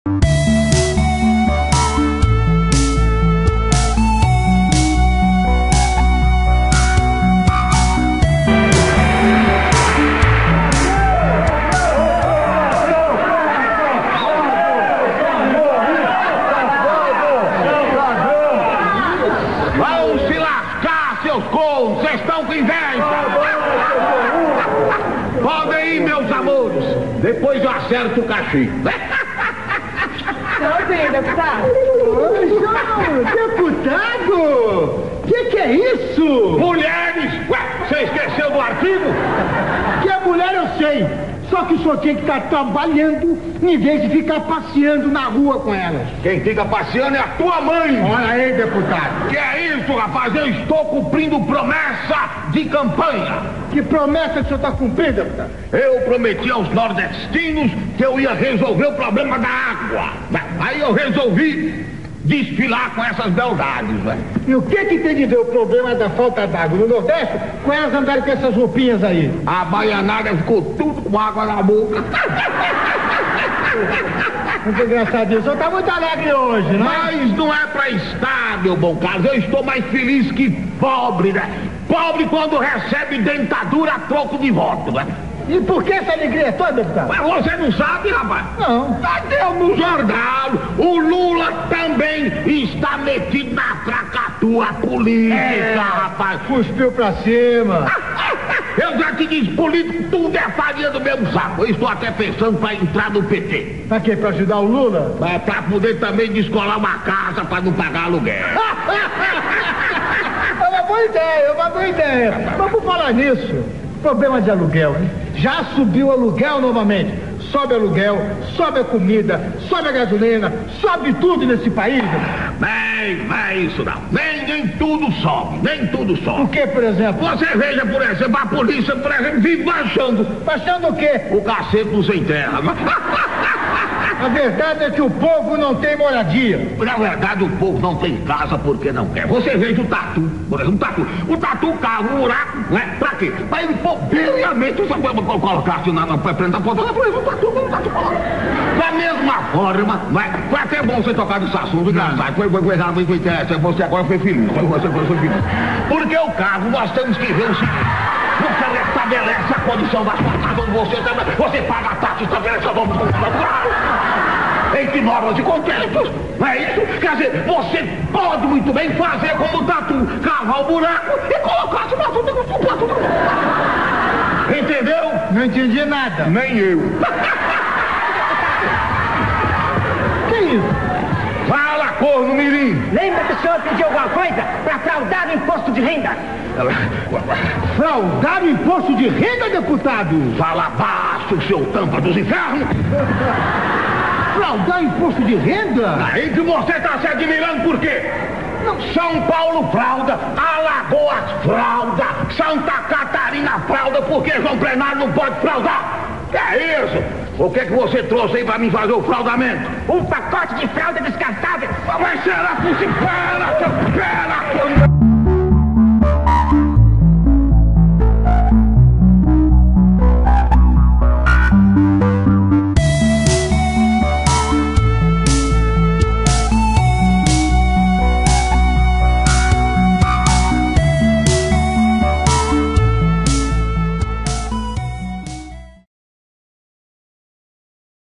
Vídeo antigo (2011) do Deputado João Plenário, já fazendo maracutaia
Sorria Gente! Esse vídeo é um pouco antigo (2011), mas parece tão atual... É porque a nossa política sempre foi uma verdadeira "palhaçada"... ainda bem que temos João Plenária para podermos rir ao invés de chorar!